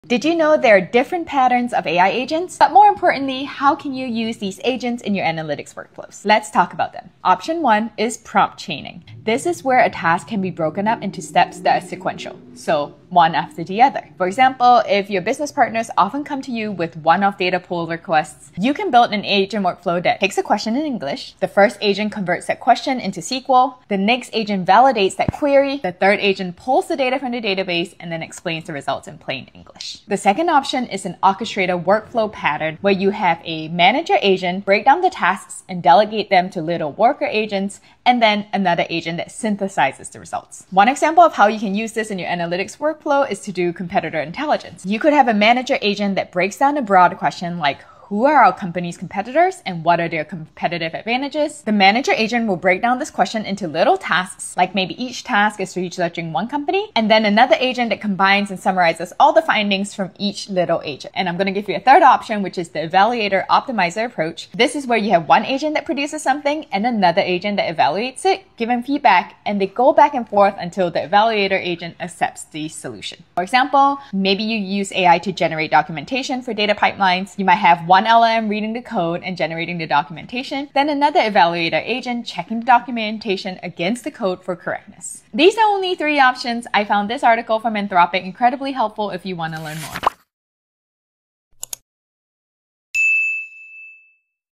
AI AGENT PATTERNS FOR ANALYTICS
Different patterns for AI agents & how to use them in Analytics:

🔗 Prompt Chaining
Break one big task into smaller, sequential steps.

Example workflow for data pulls:
- Convert question → SQL
- Validate the query
-Run query + pull data
-Explain results in plain English

🧠 Orchestrator-Workflow

A “manager” agent breaks down a big question → delegates to worker agents → combines the results.

Example: competitor intelligence research (one agent per competitor, one agent summarizing).

✅ Evaluator-Optimizer

One agent generates, another critiques → repeat until it’s right.

Example: auto-generating data pipeline docs, evaluator checks them against code before approval.

There are even more patterns.

I loved Anthropic’s article on this if you want to go deeper — go check it out